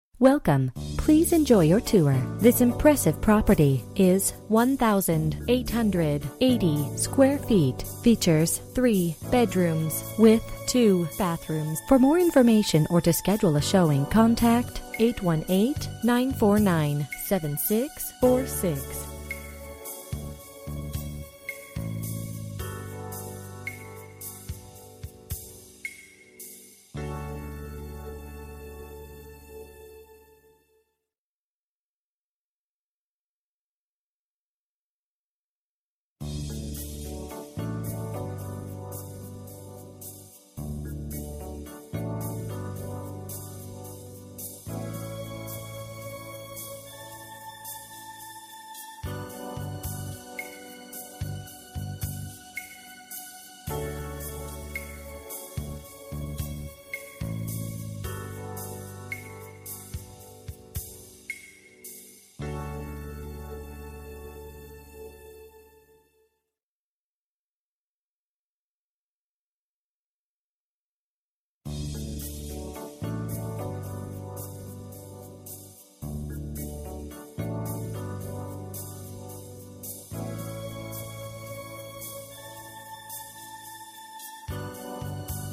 Residential for rent - 2710 MOUNTAIN PINE Drive, La Crescenta, CA 91214
This Pinecrest Atrium  style home has been updated and freshly painted.  Features - solid white oak floors, recessed low voltage lighting, architecturally reimagined kitchen, wine refrigerator, new stove, new refrigerator, prewired surround sound.  Beautiful views. Blue Ribbon Schools.

Bedrooms: 3
Bathrooms: 2 Bathrooms
Square Feet: 1880
Price: $4,200